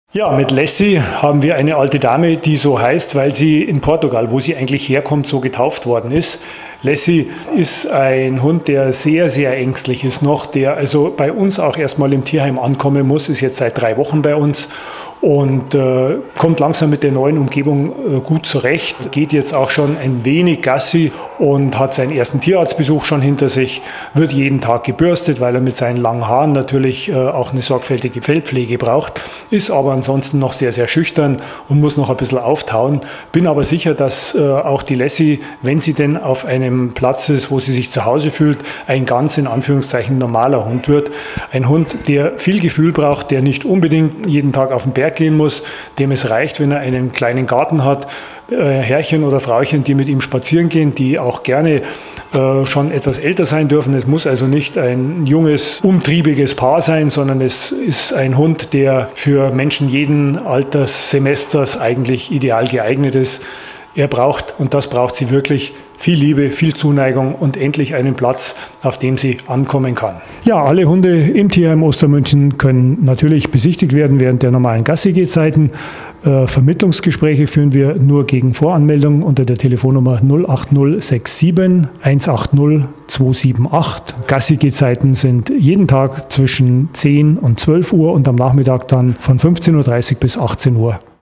Tierheim Ostermünchen: Hündin Lassie sucht ein neues Zuhause
Das ist Lassie - die liebe Hündin wohnt derzeit noch im Tierheim in Ostermünchen und würde sich sehr über ein neues Zuhause freuen. Du möchtest Lassie aufnehmen? Dann melde dich beim Tierheim Ostermünchen unter der Telefonnummer 08067/180278. Mehr Infos findest Du auf unserer Startseite